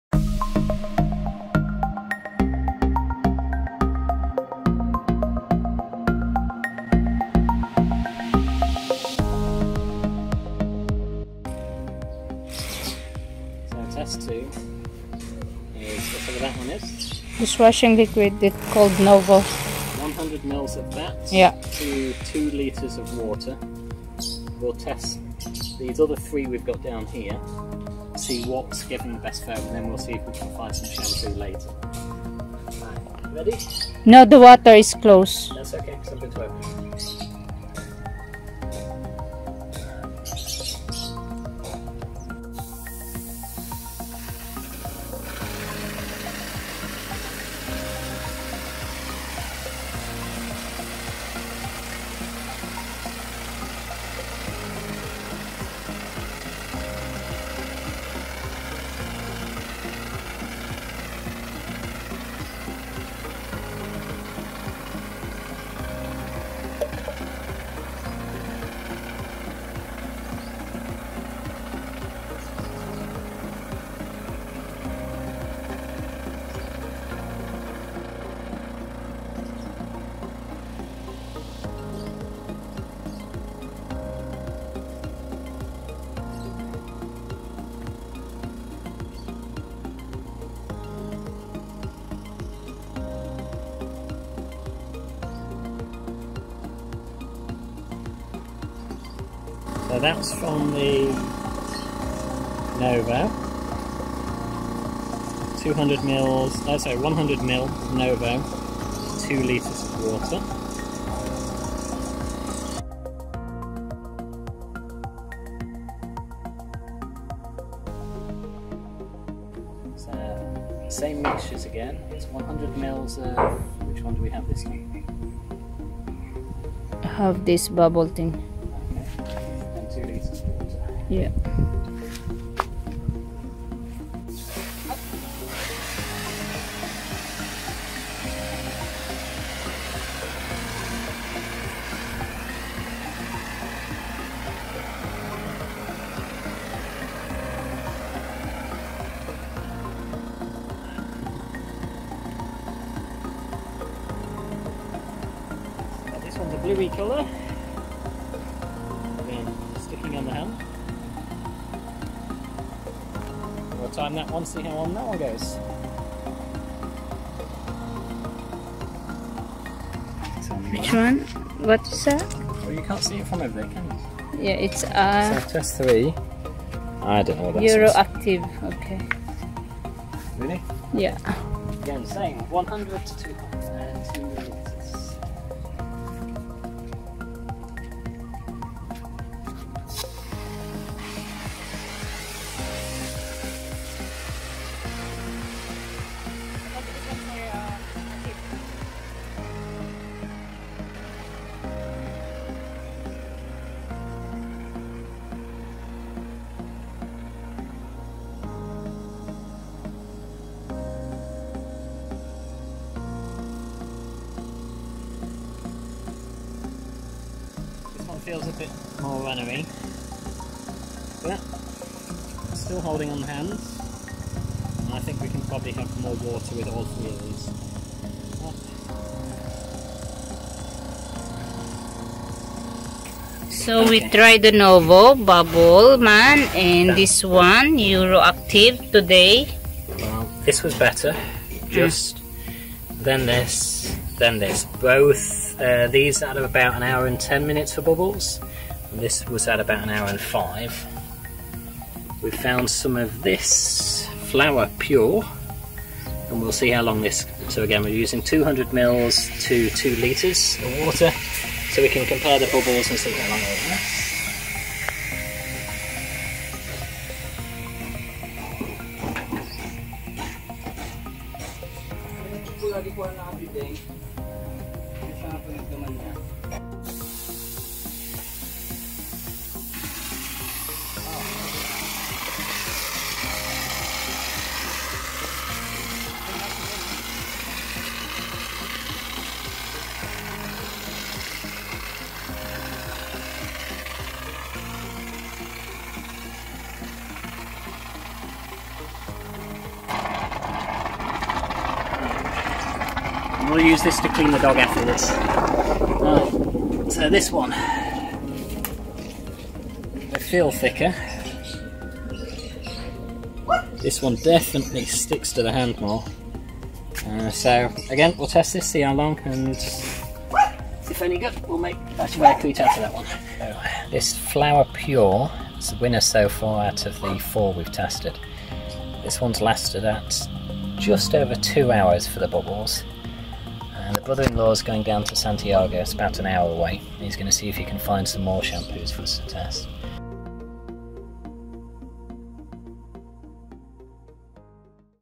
Testing some cheap shampoo and soap to use with Aircrete
As we are in the Philippines we cannot get hold of the usual products for aircrete

So we have tested a few cheap products.
The best was Flower Pure, about 2 hours holding foam
Novo, 1 hour 10 mins
Bubble Man, 1 hour 10 mins
Aero Active, 1 hour

The flower Pure is 1.86 PHP for 850ml

A big thanks to the likes of
Aircrete Harry
Honey Do Carpenter
Aircrete Creations
and others your ideas and plans are what have me trying new ideas on our land